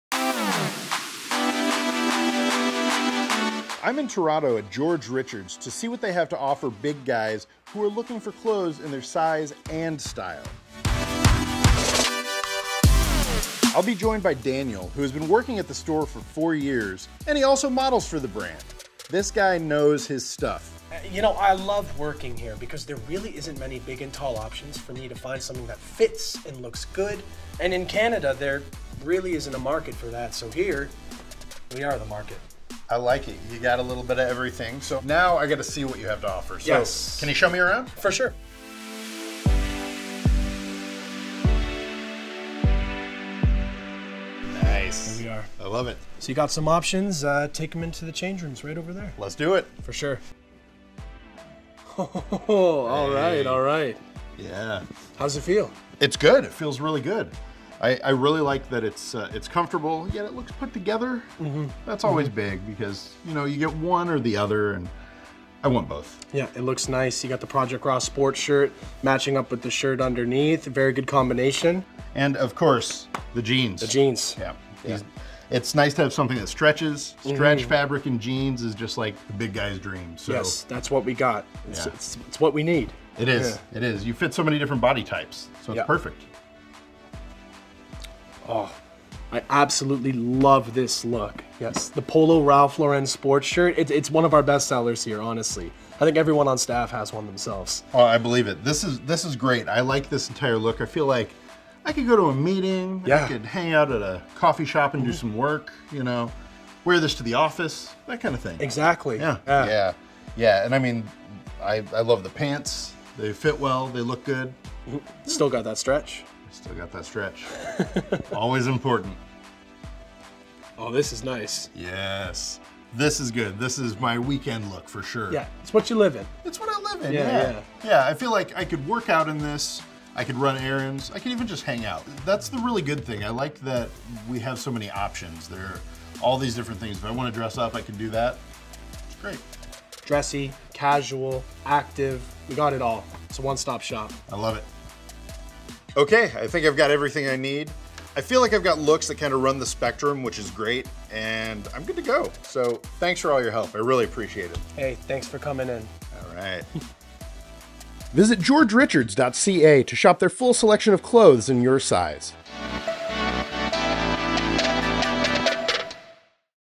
George Richards: Behind the Brand
We visit a George Richards store in Toronto, Canada, to find out what they have to offer for big & tall guys. While there, we meet Daniel, a Style Expert at the store and a model for the company!

Read our article about George Richards at Chubstr.com